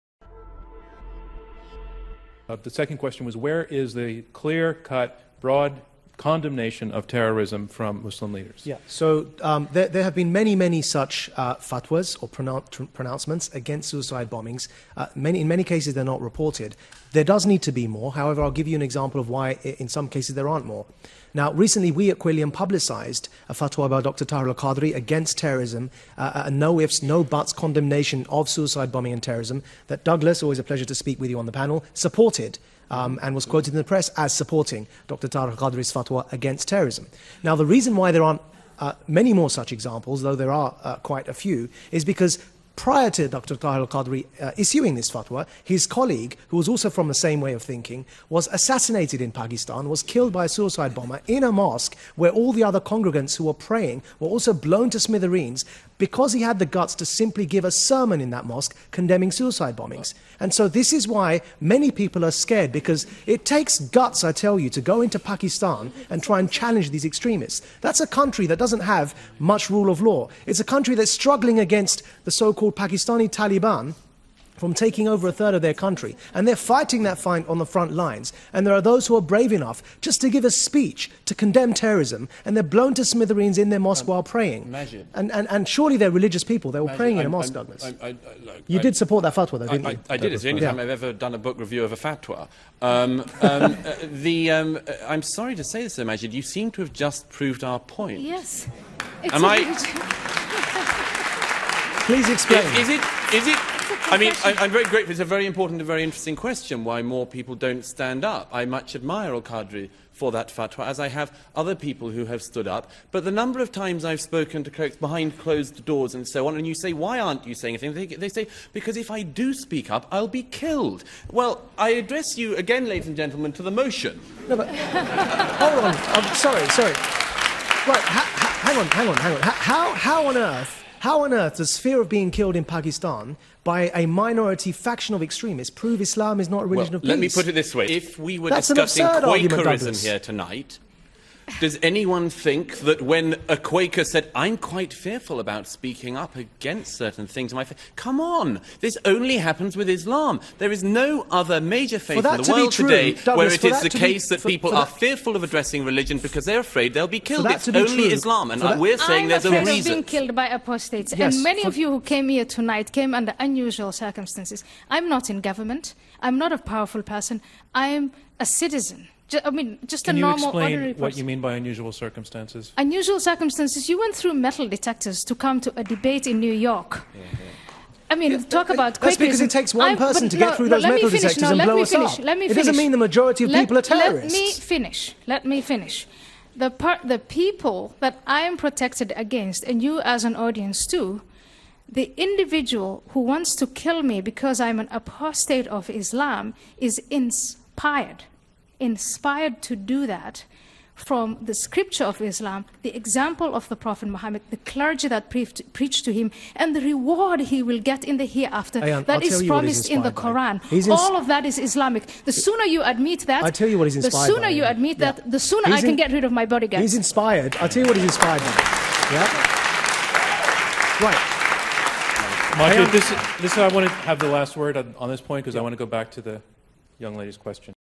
Islamic Advocate Crushed Proving Islam Is Peaceful
Islamist Apologist Shoots Himself In the Foot Trying To Prove Islam Is Peaceful